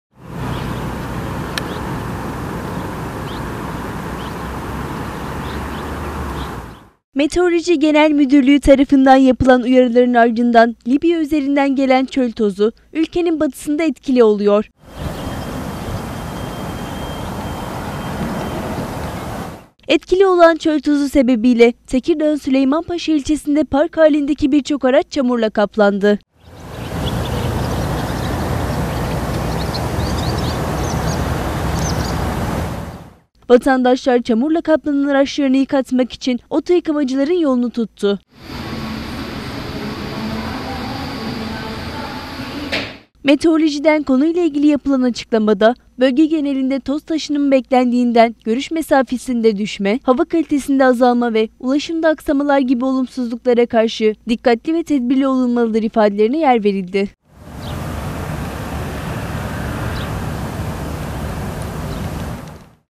ÇÖL TOZU ALARMI: ÜLKENİN BATISINI SARIYOR
Libya üzerinden gelen çöl tozu, ülkenin batısını etkisi altına aldı.